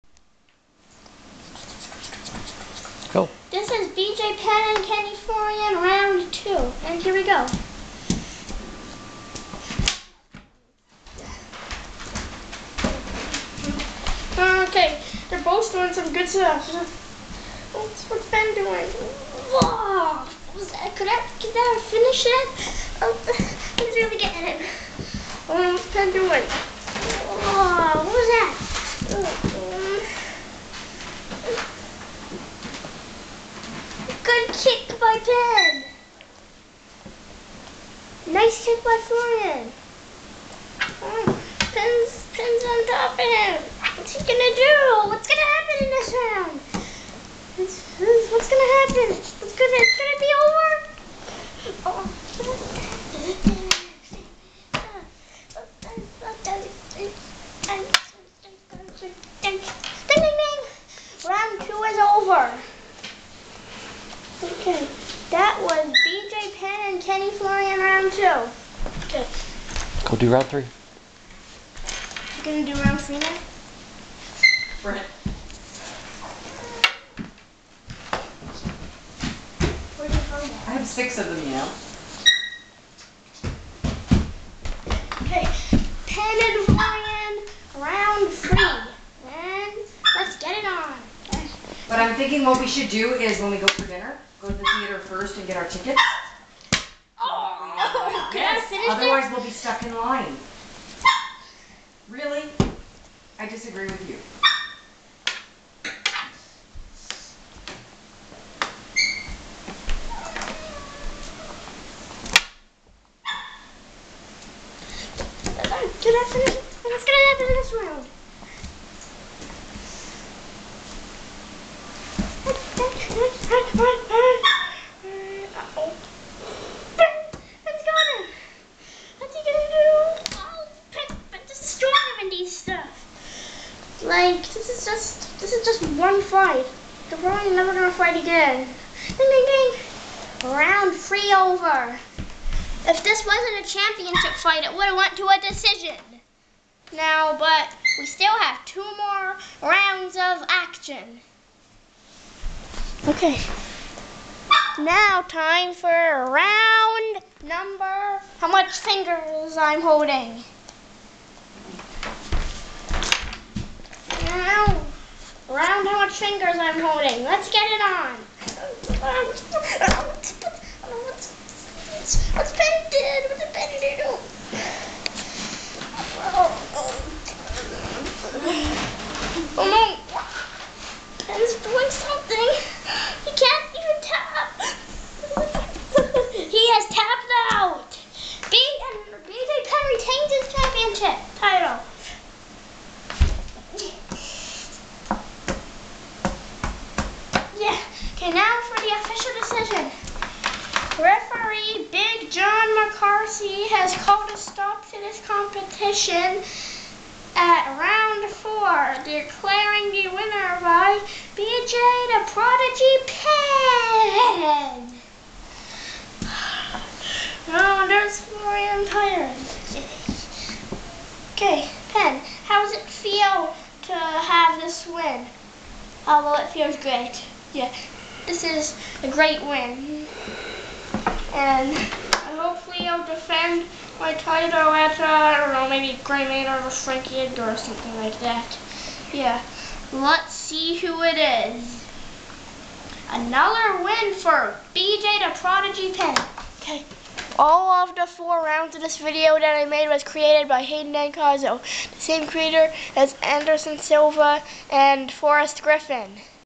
W.U.F.C Webkinz fight (B.J Penn VS Kenny Florian)
I was board so I got some Webkinz and made them fight. W.U.F.C stands for Webkinz Ultimate Fighting Championship LOL

so here"s who the fighters really are

Numy(Kenny Florian)
Kurt(B.J Penn)